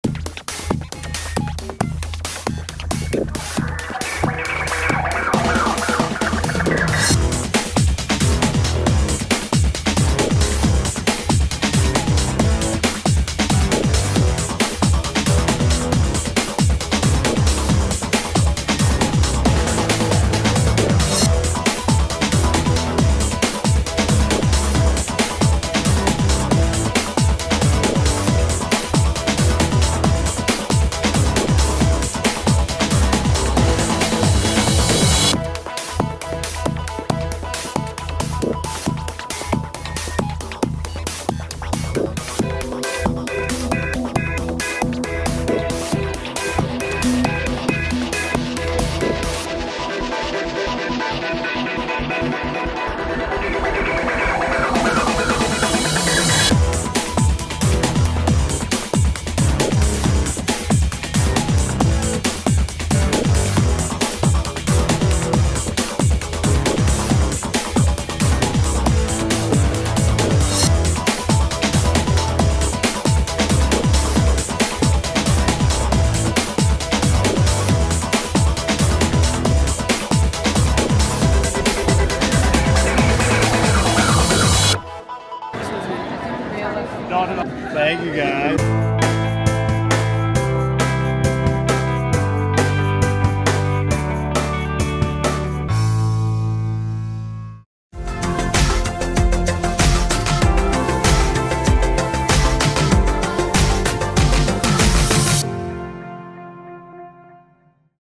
Calgary Expo April 26, 2024 Featuring Skeet Urich from Scream!
Drove down to Calgary to the Calgary Expo featuring the cast of Scream!  Waited to meet Skeet Ulrich for someone's birthday and after they had their moment he asked me about the camera I had and showed him some of the features of the Osmo Pocket 3.